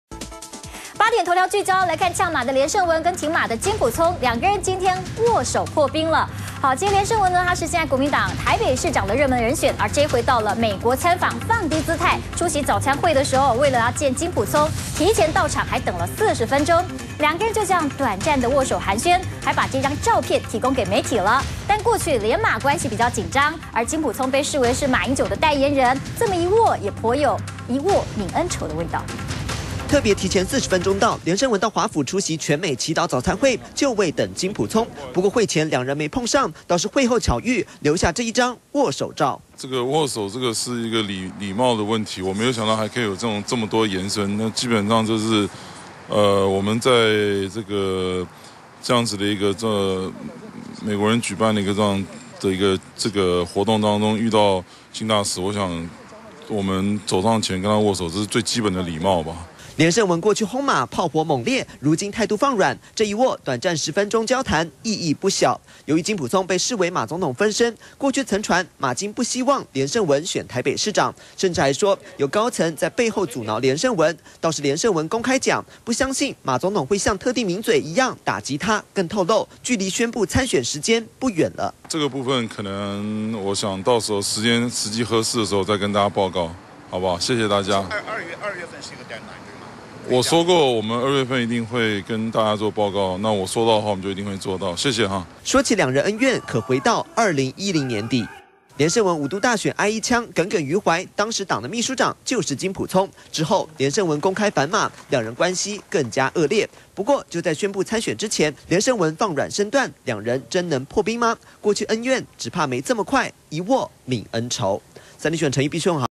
破冰？連勝文、金溥聰巧遇握手寒喧｜三立新聞台
國民黨台北市長熱門人選連勝文，跟即將返台接任國安會秘書長的馬總統核心幕僚金溥聰，在華府出席祈禱早餐會時簡短的握手寒暄，連勝文辦公室也發出1張2人握手的照片給媒體。根據了解，連勝文還特地提早40分鐘到會場要見金溥聰，不過一直到餐會即將結束時2人才終於碰面，而過去連馬關係緊張，這麼一握也頗有一握泯恩仇的味道。